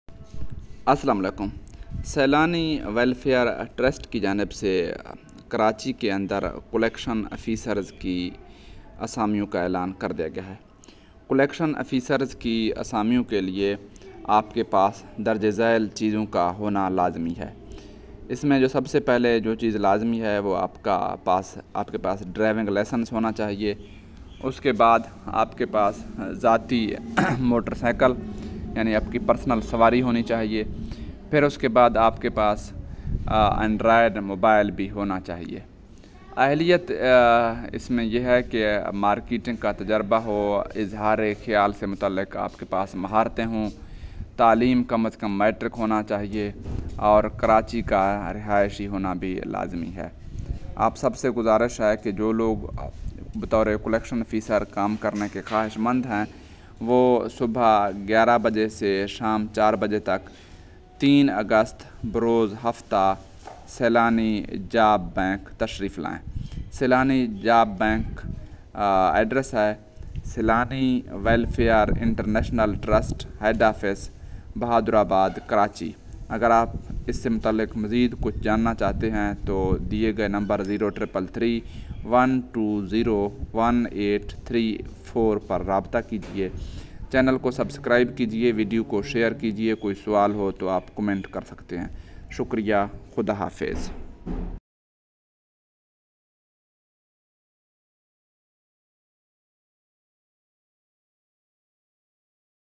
Saylani welfare Trust Jobs in Karachi 2024 | Jobs for matric pass degree holder
Learn Saylani welfare Trust Jobs in Karachi 2024 . Collection officer Jobs for matric pass degree holder are available.
Submit your application through the mentioned procedure.